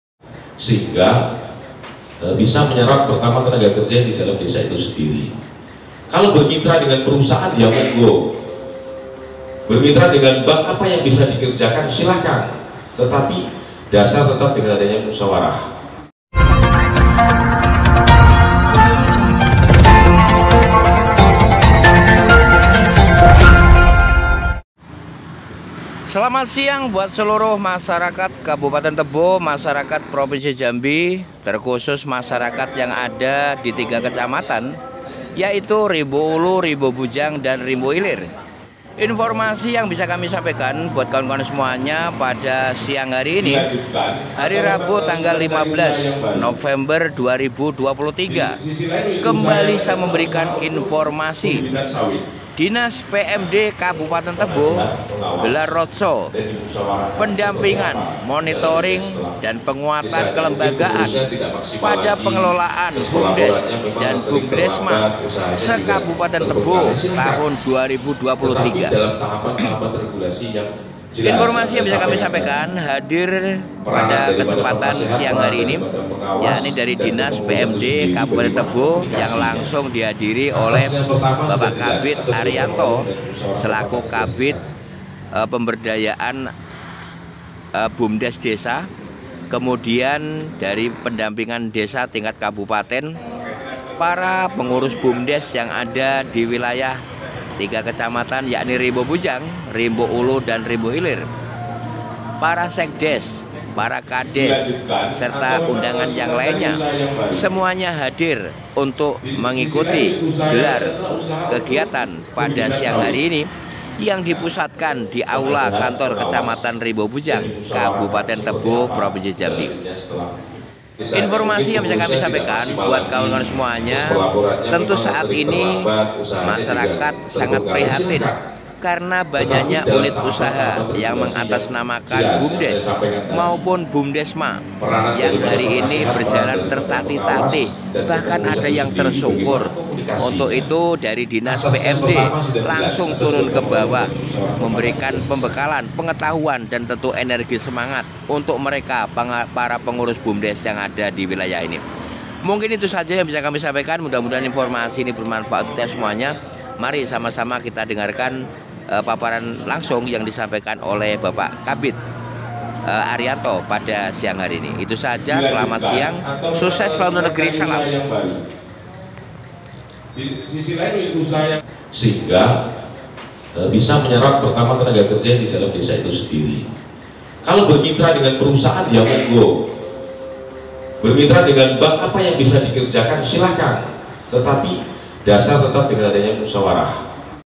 Dinas PMD tebo gelar Road Show pendampingan,monitoring BUMDES di Rimbo bujang,ini agendanya
Road Show pendampingan,monitoring dan penguatan kelembagaan pada pengelolaan bumdes/BUMDESMA kabupaten tebo 2023